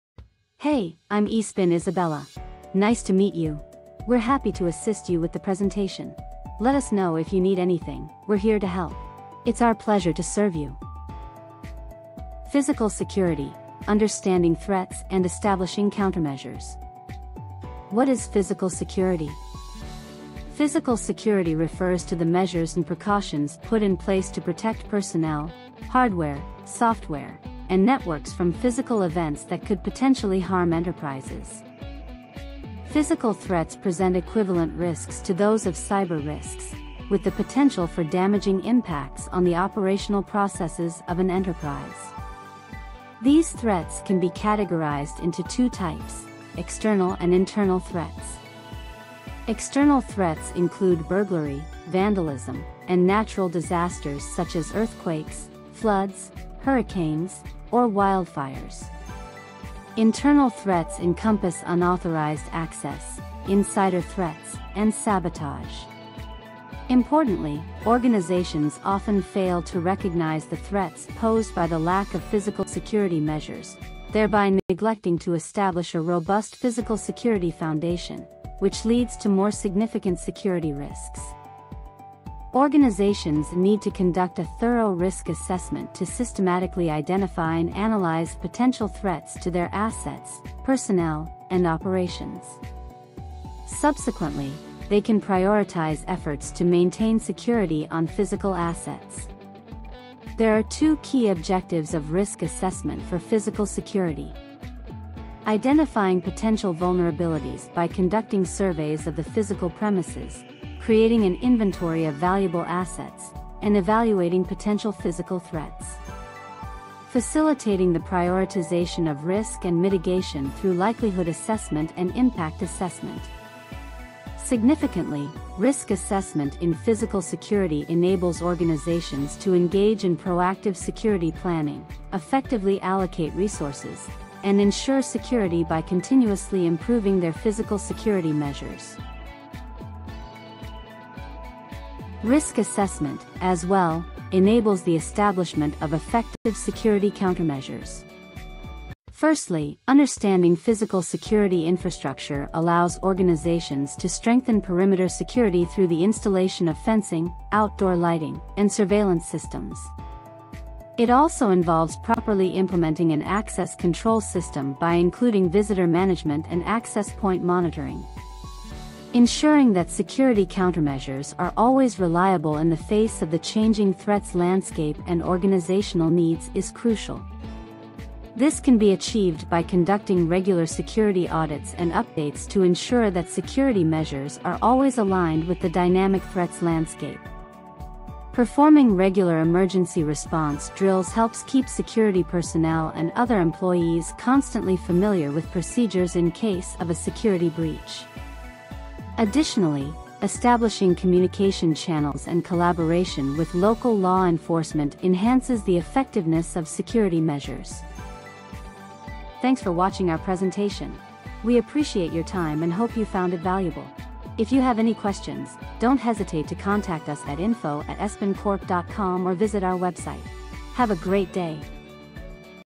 Physical Security Understanding Threats & Establishing Countermeasures
This video excerpt is from the E-SPIN Security Awareness Training (SAT). It discusses Physical Security regarding physical threats, risk assessment and security countermeasures. For a complete syllabus or customization for your enterprise, including additional modules, please reach out to E-SPIN.

Chapter:
0:12 Physical Security: Understanding Threats & Establishing countermeasures
0:19 What is Physical Security?
0:32 Understanding Physical Threats
1:20 Risk Assessment for Physical Security
2:21 Physical Security Countermeasures
3:36 Contact us